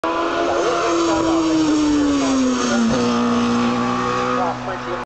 Richard Gaylard Force 2012 Gurston Down R2
Richard Gaylard's Force blasts through Hollow Bend at around 110mph at Gurston Down, 22nd July 2012.